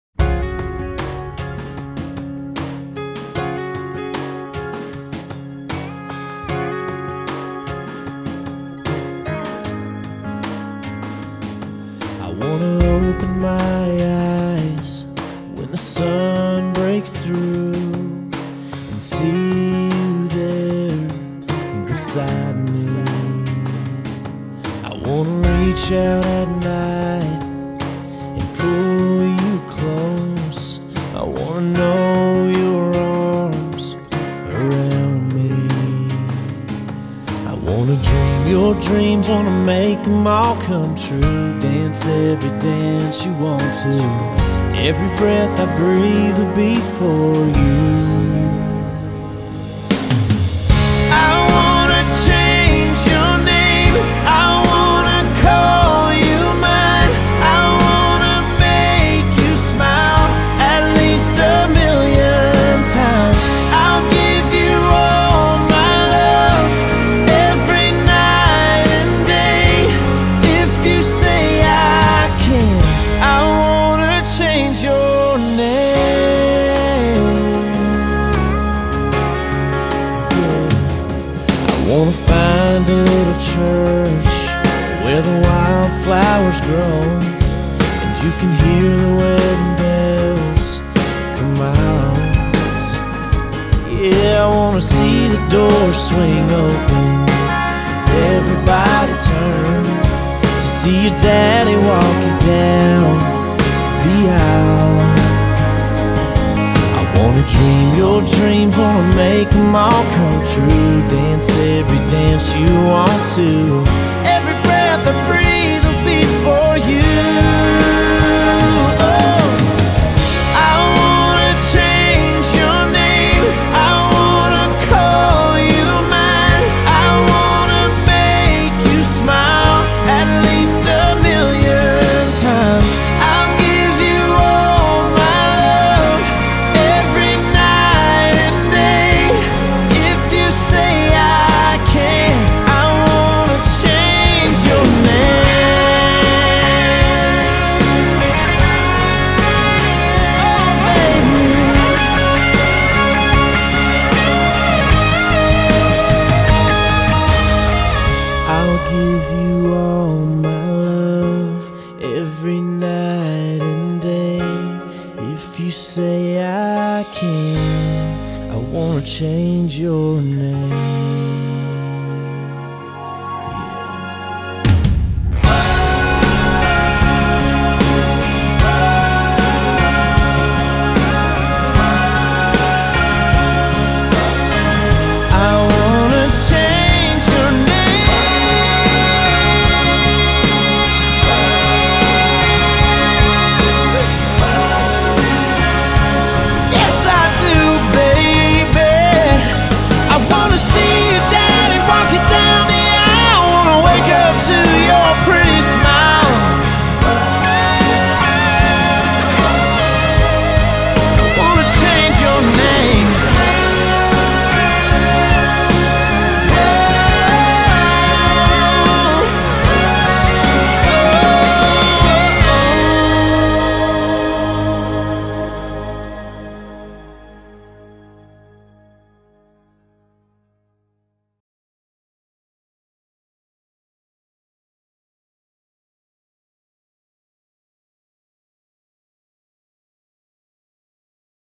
Frensham Heights School Wedding Video
Jennifer and Nathan's wedding highlights video at Frensham Heights School